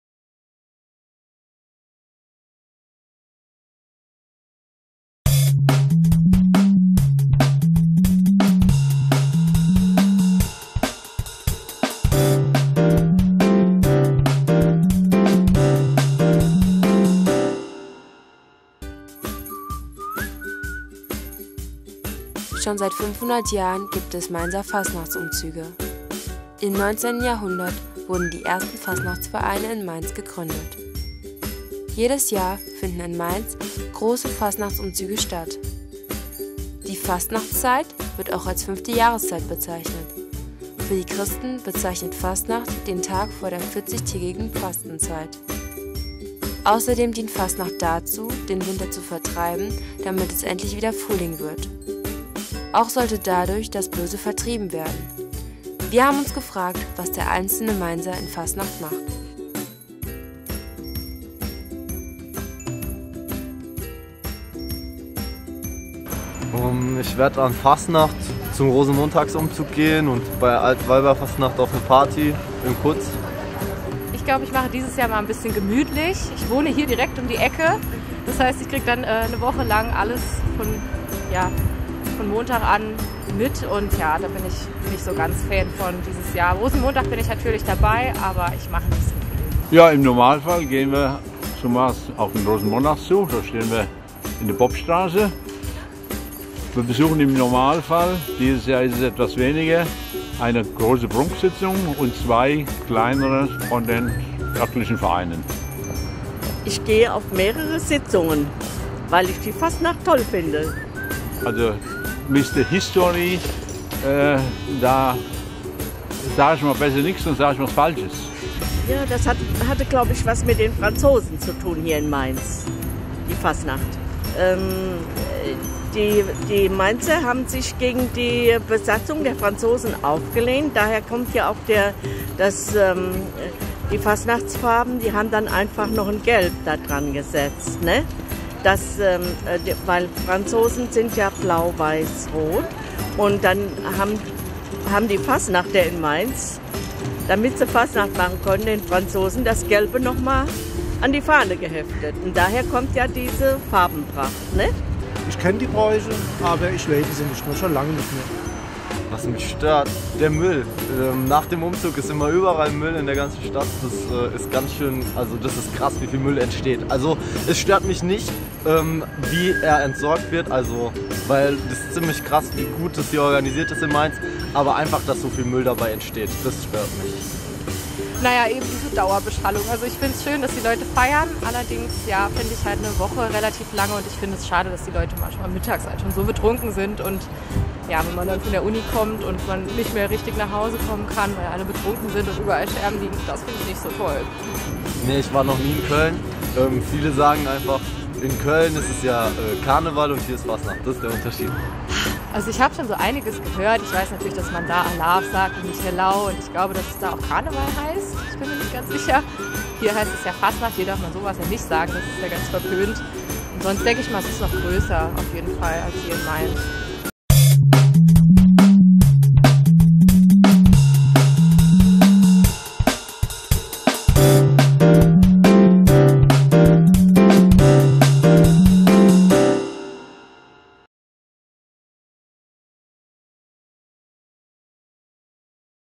Fastnacht 2015 | Mainzer Meinung
Wir haben uns gefragt was der einzelne Bürger an Fastnacht macht, welche Bräuche er kennt und ob er über die Hintergrüde des Festes Bescheid weiß.